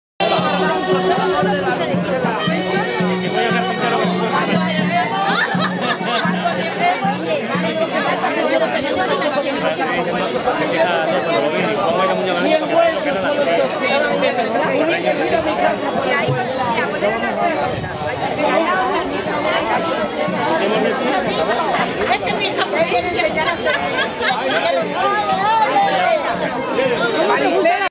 Día de la Era en Muñogalindo 2
Muñogalindo (Ávila) ha celebrado el 11 de agosto de 2012 la quinta edición del Día de la Era, con demostraciones de trilla y lavado de ropa a mano, entre otras antiguas labores.